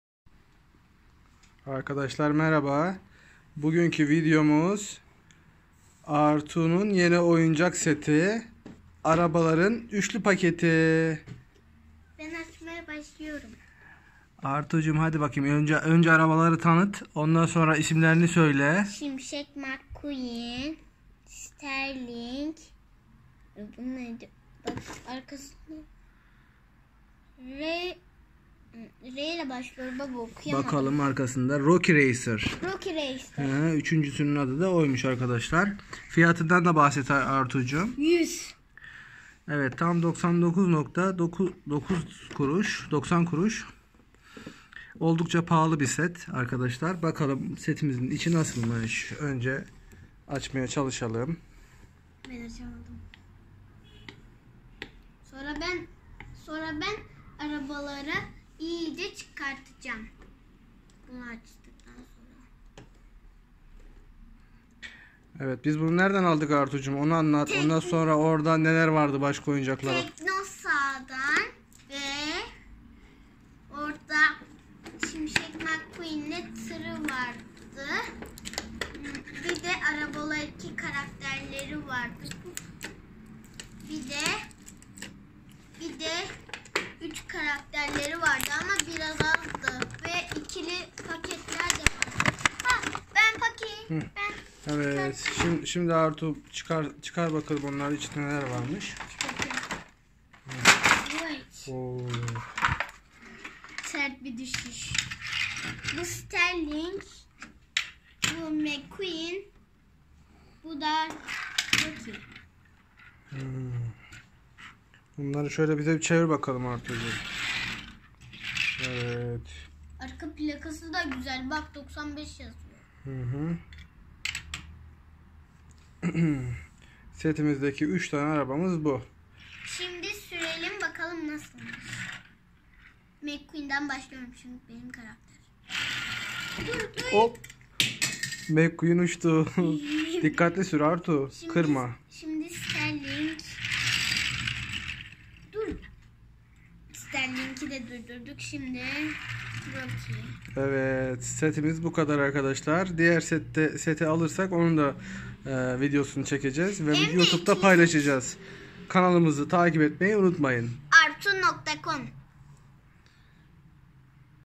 Cars3 Büyük boy araba kutu açma
Bu videomuzda Artuğ ile Arabalar 3 Büyük boy araçları açıyoruz.
kutu içeriğinde;
*Şimşek Mc Quin
*Sterling
*Rokie Racer araçlarının tanıtımını yapıyoruz
keyifli seyirler...